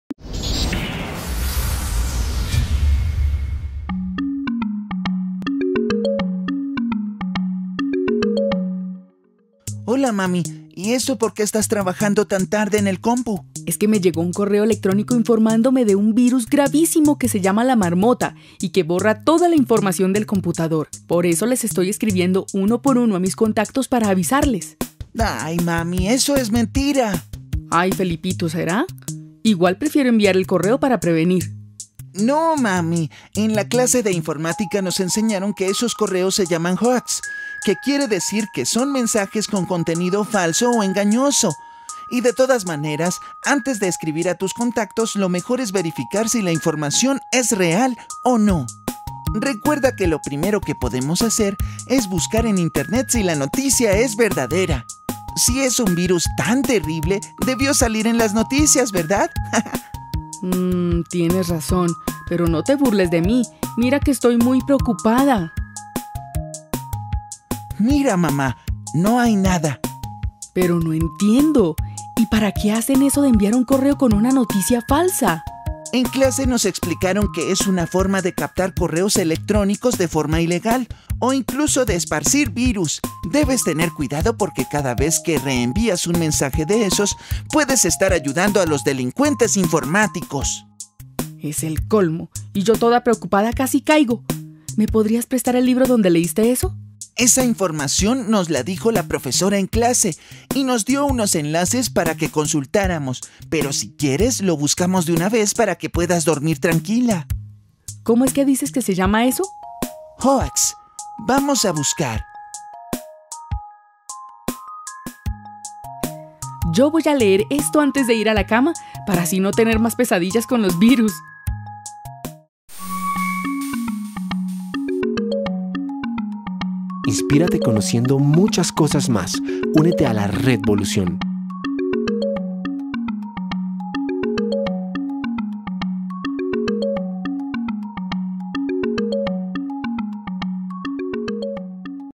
Noticias falsas en la red.
con esta unidad de contenido, aprenda como reconocer cuando una noticia es falsa y a conocer los principales casos de noticias falsas en la red.